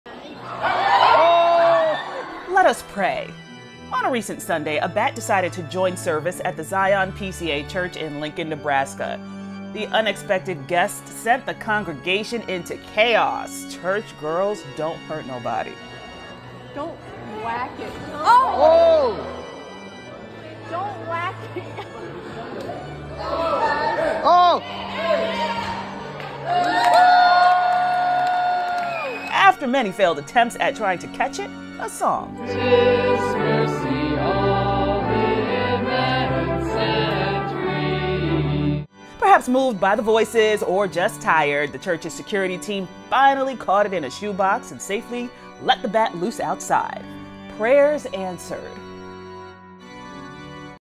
Bat Shows Up to Church Service
A bat causes chaos during a service at a church in Lincoln, Nebraska.